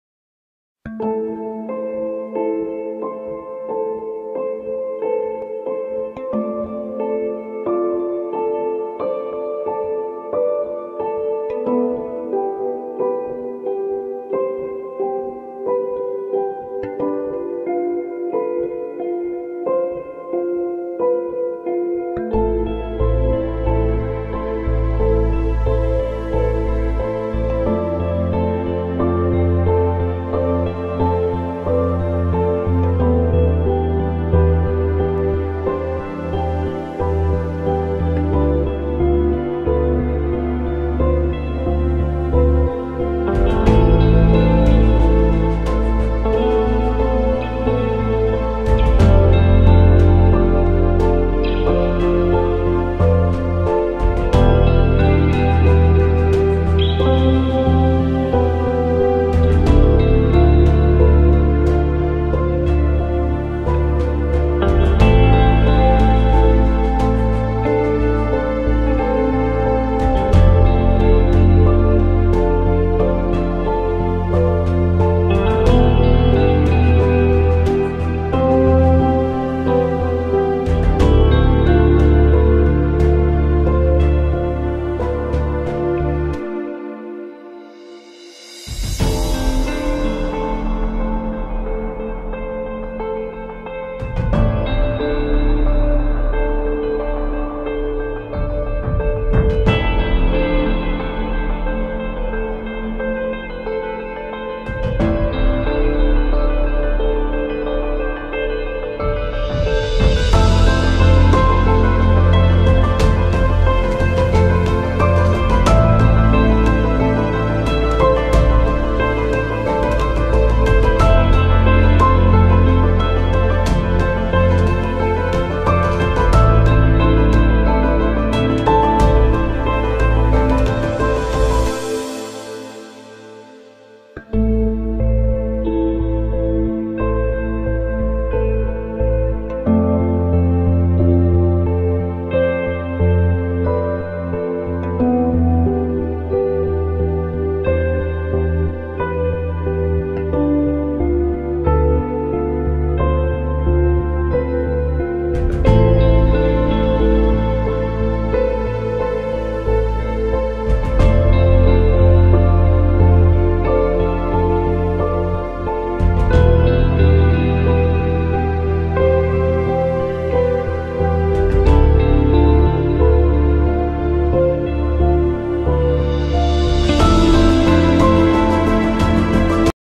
A View of a Farmer ~ Cultivating the Cornfield!🚜 ~ HD Drone Footage
In this video you see us cultivating the field where the corn has grow and harvested. We seeding now yellow mosterd for the winter. We are cultivating the field crooked because there a alot of tracks in the field. In the spring we plow the yellow mosterd and it digested in the ground.